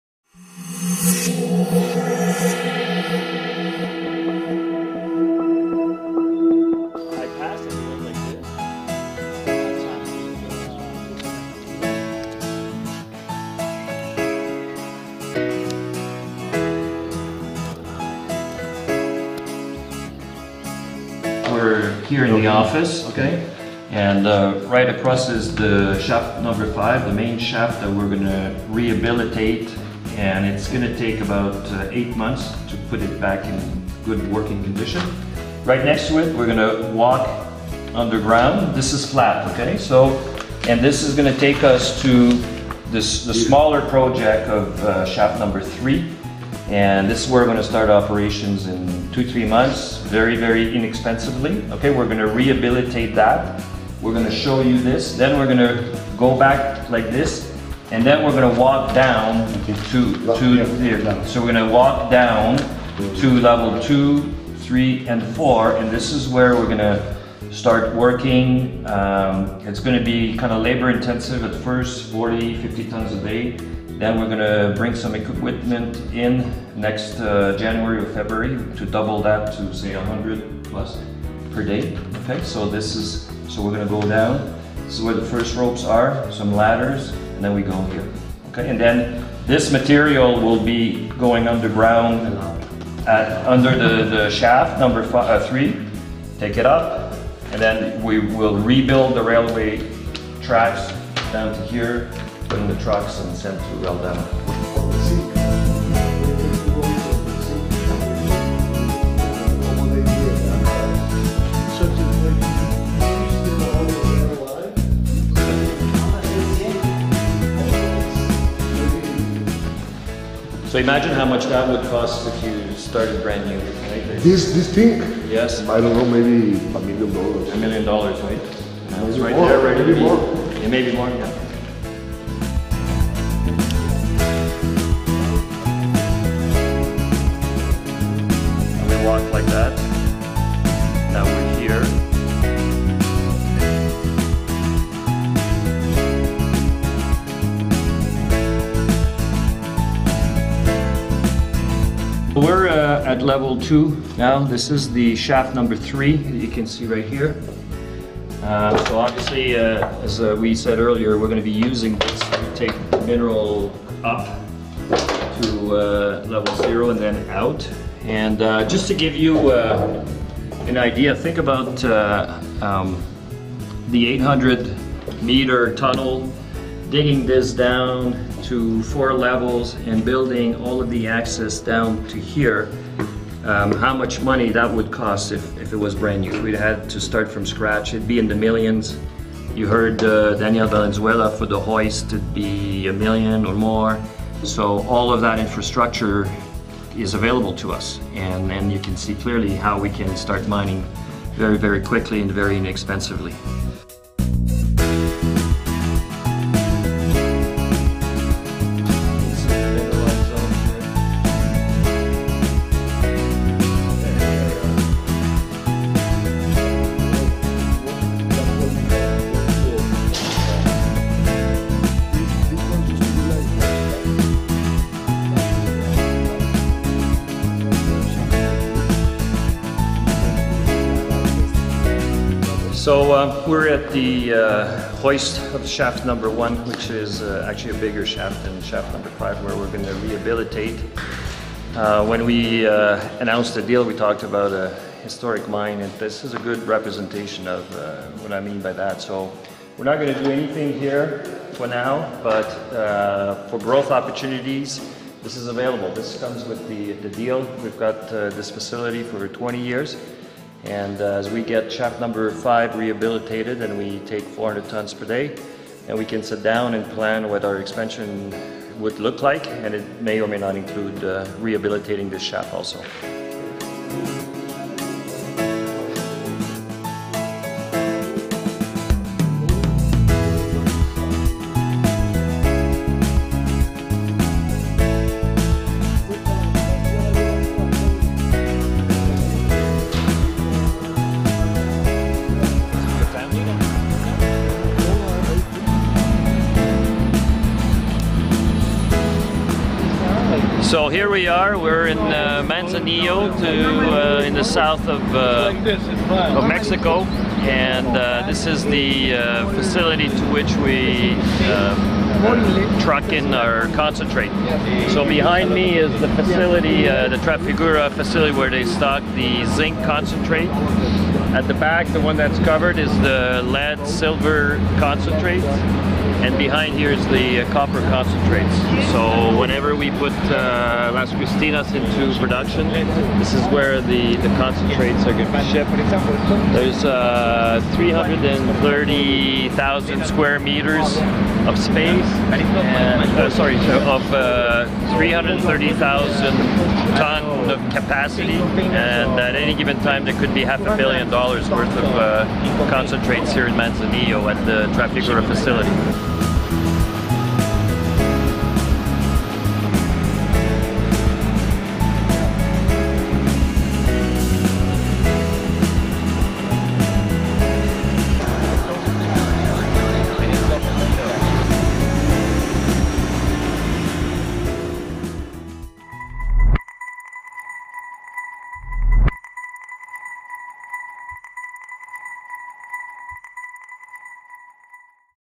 Commodity-TV: Sitevisit Cyprium Mining
Cyprium Sitevisit Of Potosi Silver Mine & Trafiguras Port In Manzanillo
CEO Alain Lambert Guides The Tour Through The Facilities